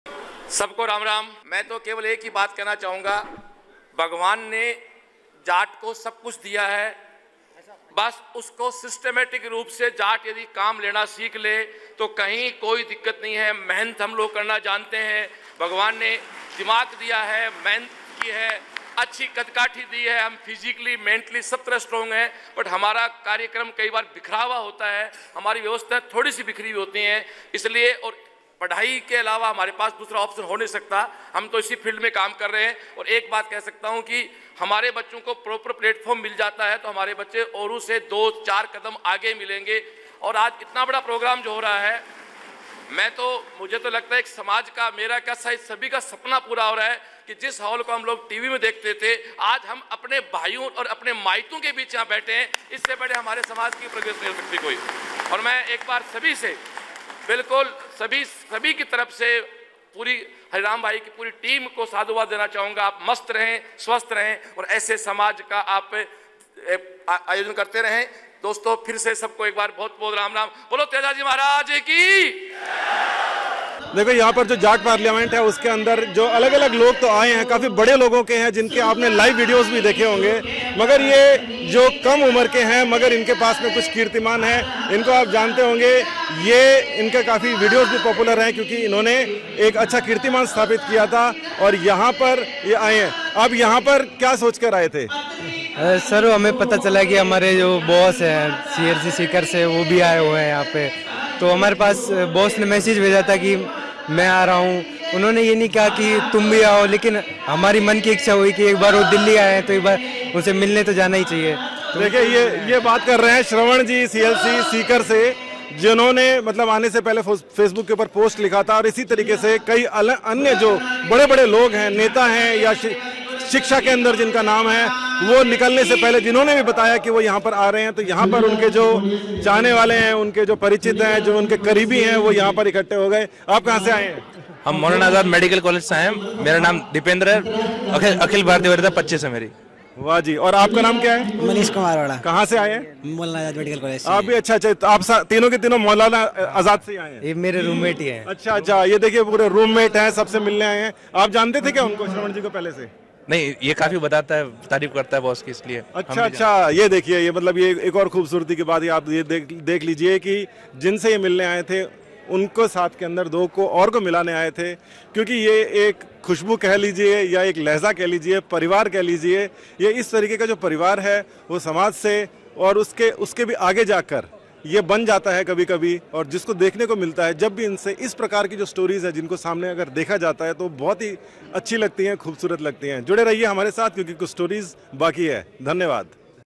Director CLC, Shrawan Choudhary at Vigyan Bhawan
Director CLC Shrawan Choudhary addressing conference at Vigyan Bhawan